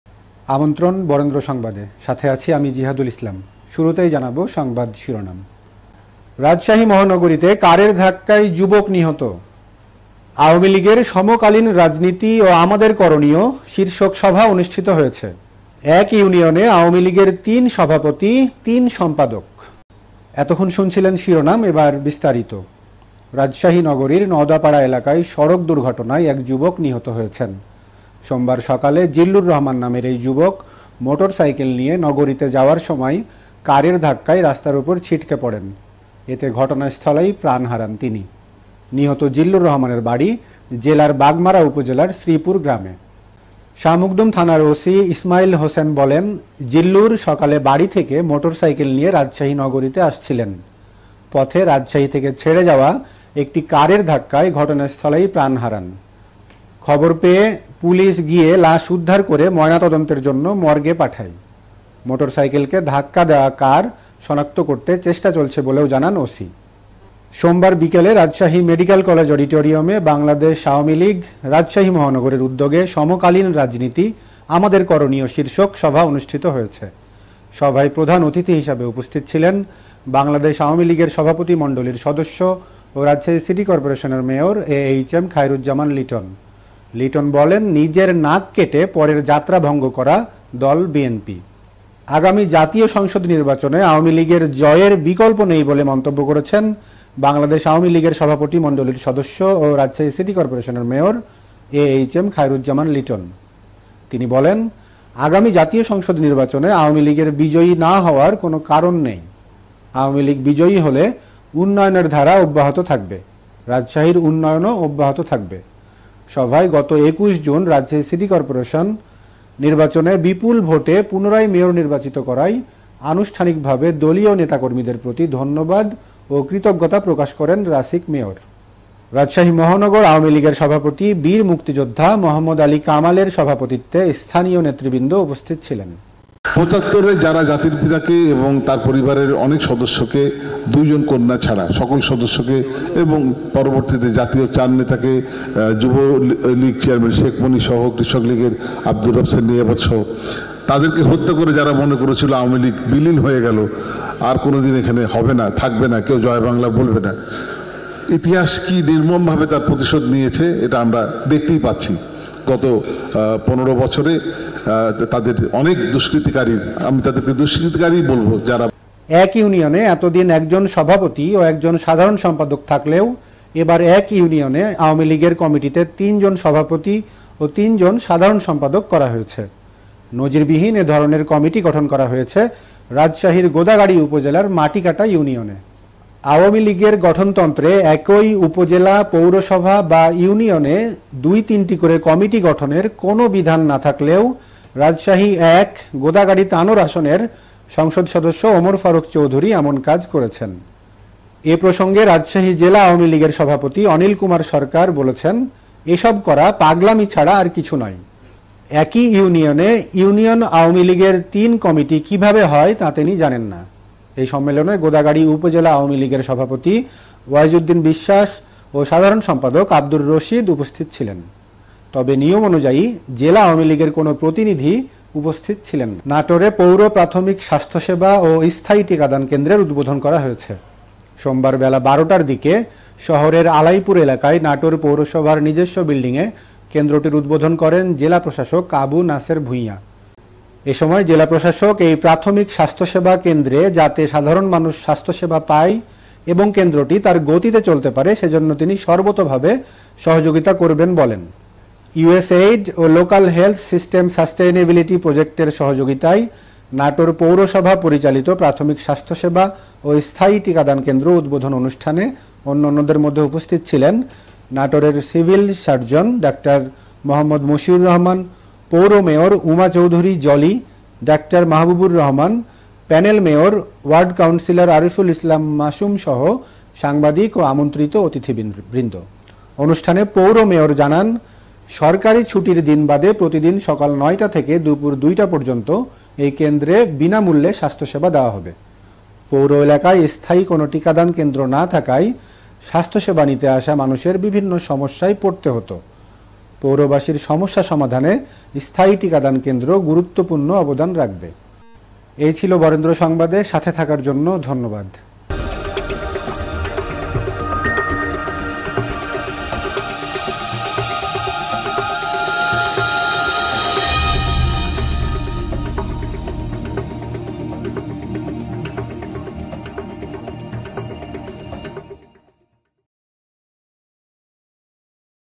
এক ইউনিয়নে আ.লীগের তিন সভাপতি তিন সম্পাদক | এমপি ফারুক চৌধুরীরর নতুন রাজনৈতিক চমক | বিষ্ময়
এক ইউনিয়নে আ.লীগের তিন সভাপতি তিন সম্পাদক

এক ইউনিয়নে এতদিন একজন সভাপতি ও একজন সাধারণ সম্পাদক থাকলেও এবার এক ইউনিয়ন আওয়ামী লীগের কমিটিতে তিনজন সভাপতি ও তিনজন সাধারণ সম্পাদক করা হয়েছে। নজিরবিহীন এ ধরনের কমিটি গঠন করা হয়েছে রাজশাহীর গোদাগাড়ী উপজেলার মাটিকাটা ইউনিয়নে।

আওয়ামী লীগের গঠনতন্ত্রে একই উপজেলা, পৌরসভা বা ইউনিয়নে দুই-তিনটি করে কমিটি গঠনের কোনো বিধান না থাকলেও রাজশাহী-১ (গোদাগাড়ী-তানোর) আসনের সংসদ সদস্য ওমর ফারুক চৌধুরী এমন কাজ করেছেন। এ প্রসঙ্গে রাজশাহী জেলা আওয়ামী লীগের সভাপতি অনিল কুমার সরকার বলেছেন, এসব করা পাগলামি ছাড়া আর কিছু নয়। একই ইউনিয়নে ইউনিয়ন আওয়ামী লীগের তিন কমিটি কিভাবে হয় তা তিনি জানেন না। এ সম্মেলনে গোদাগাড়ী উপজেলা আওয়ামী লীগের সভাপতি অয়েজুদ্দিন বিশ্বাস ও সাধারণ সম্পাদক আব্দুর রশিদ উপস্থিত ছিলেন। তবে নিয়মানুযায়ী জেলা আওয়ামী লীগের কোনো প্রতিনিধি উপস্থিত ছিলেন না।